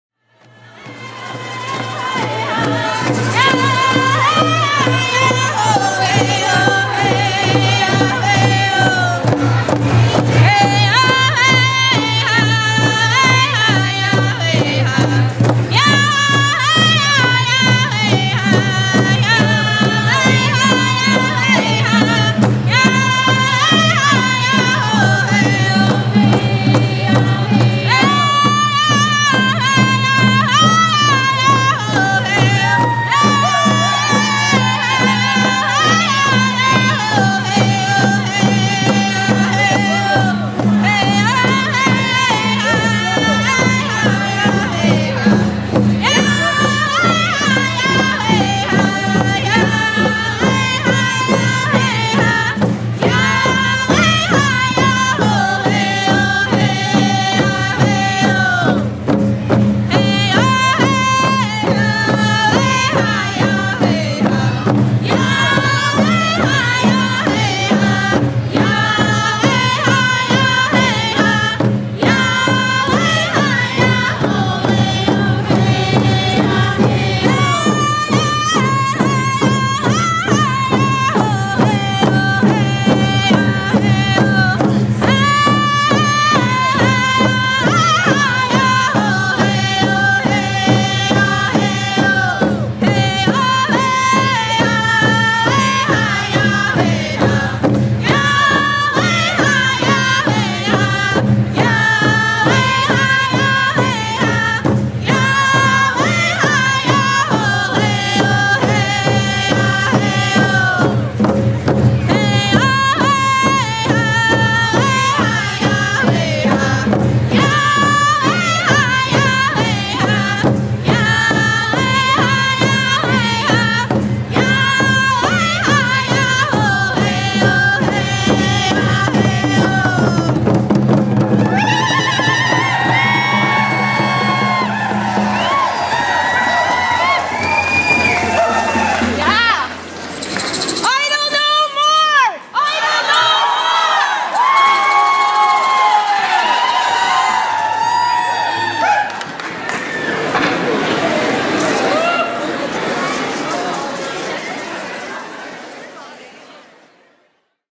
Idle No More: Flash Mob Soloist sings Honor Song
A unifying song for the Idle No More cause at the Masonville Place Mall London Ontario